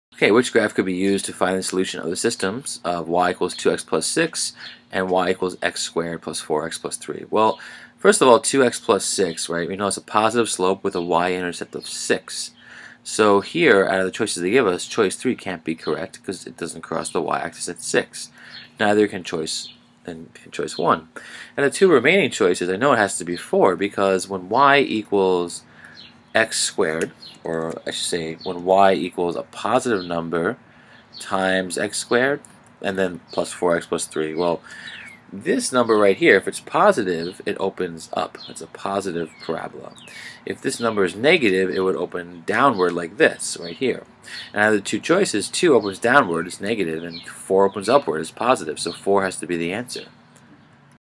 Quadratic System 3 Algebra Regents
In this video we look at the intersection between and linear and quadratic function